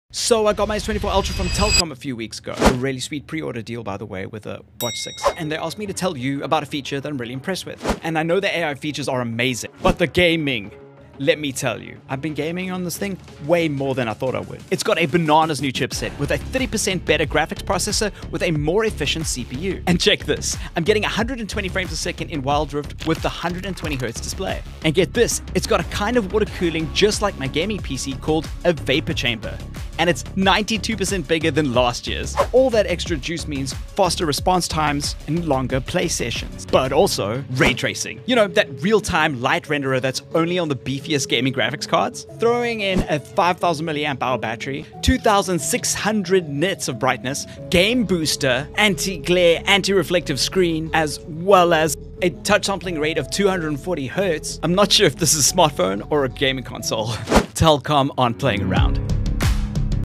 Gaming on the S24 Ultra is 🤌
The TelkomXSamsung best value partnership is here! I got asked which has been my favourite features, and one I don't see people talking about enough is it's gaming power.
👉 Screen: This screen is fantastic: 2600 nits peak brightness, best-in-class anti-reflective Gorilla Armor 120hz with a 240hz touch sampling rate for faster input.
👉 Battery: 5000mAh to keep me in game for ages!
👉 Ray Tracing: Beautiful, realistically rendered light in supporting titles.
👉 Vapor Chamber: A custom vapor chamber makes the S24 Ultra cooler, with a 92% increase in size from last year.
to get your Samsung Galaxy S24 from Telkom online and join the BestValueNetwork today!